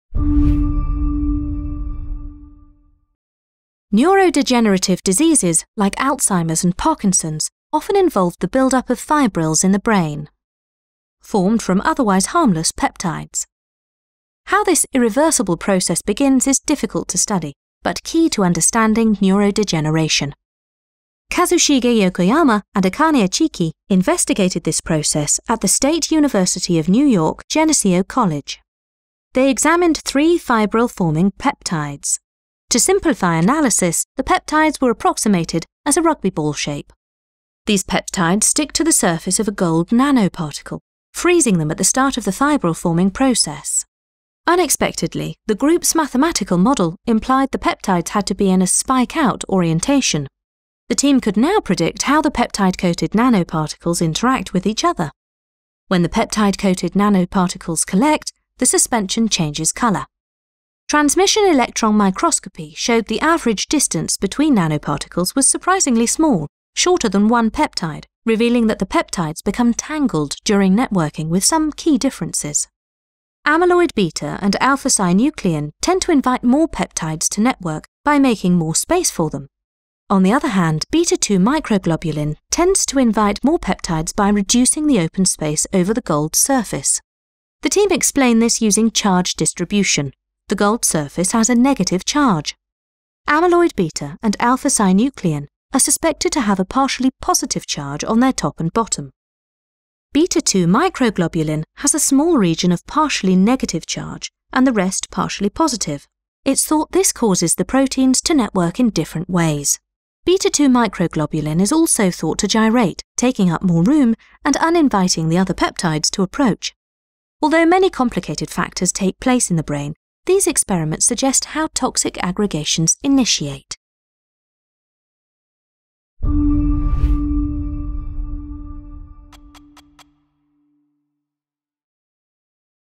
How do fibrils form?
Fibrils are implicated in neurodegenerative diseases such as Alzheimer’s and Parkinson’s. Using gold nanoparticles to ‘freeze’ peptides at the point they start to form fibrils can give us valuable insight into how these damaging particles form. This study was conducted at the undergraduate institution, State University of New York – Geneseo College, with a very limited research budget. In order to conduct a good quality study, the method was carefully designed and a good amount of time spent on the data analyses, combining a mathematical approach with chemical knowledge. Using gold nanoparticles allowed the team to demonstrate how proteins bond to each other.